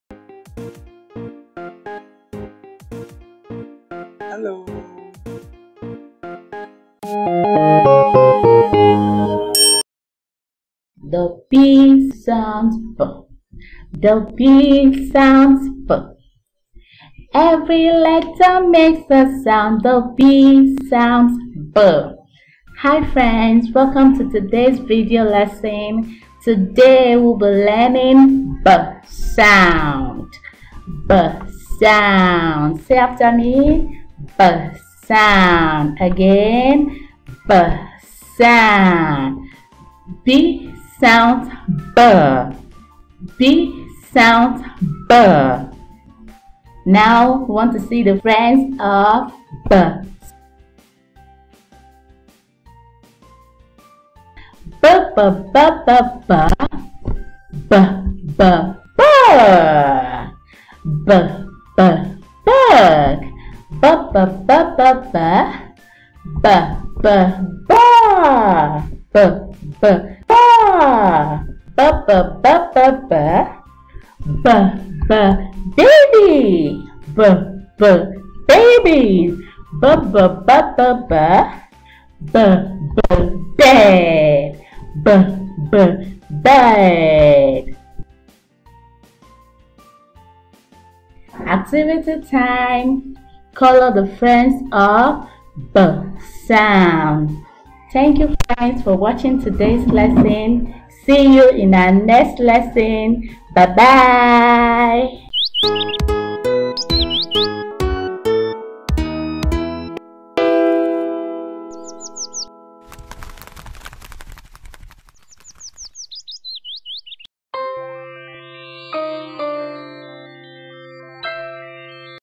B Sound
Learn about the 'B sound' and its friends. Watch and share!